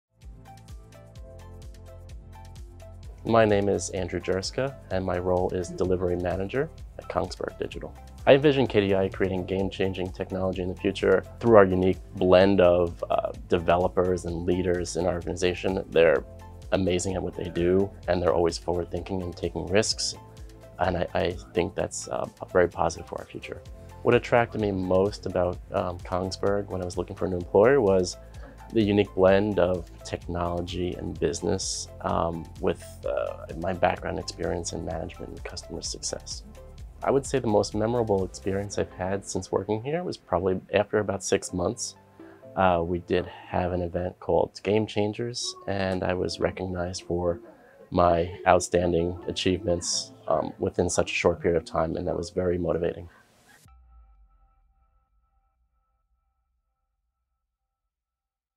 Meet the People in Kongsberg Digital ✨ Andrew Jursca
👋 Welcome to another episode of "Meet the People of Kongsberg Digital"! Today, we introduce Andrew Jursca, an Operation Manager at Kongsberg Digital in Houston. Join us as Andrew shares his vision for KDI's future technology and reflects on his unique journey blending technology, business, and customer success. 💻